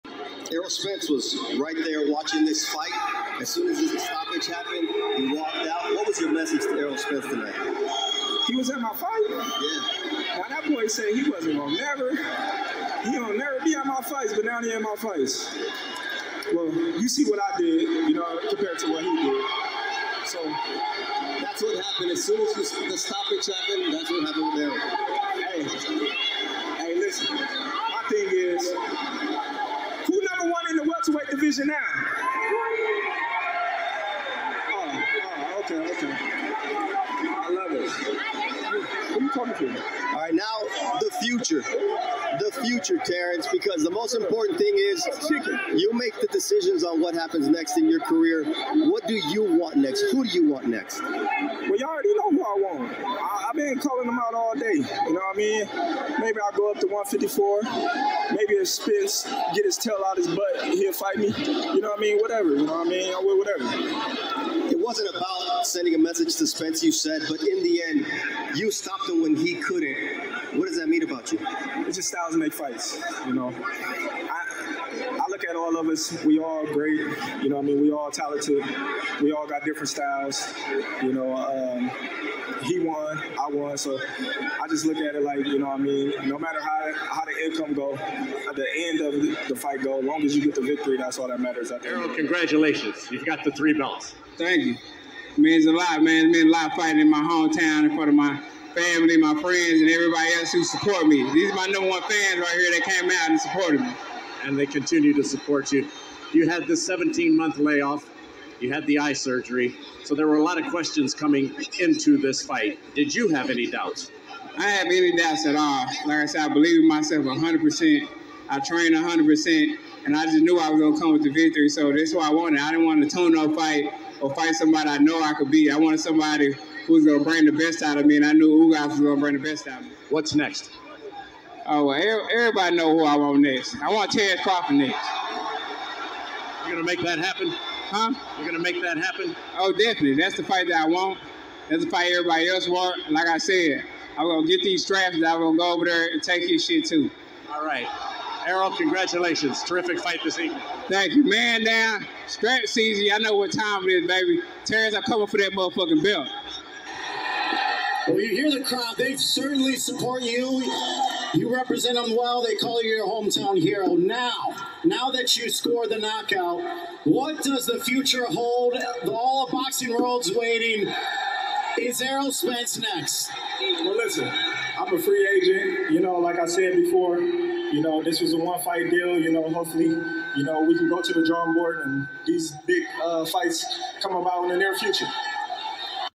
WHAT'S NEXT?? Are Crawford's sights still set on Errol Spence Jr.?
Are Crawford's sights still set on Errol Spence Jr.? listen to the difference in responses.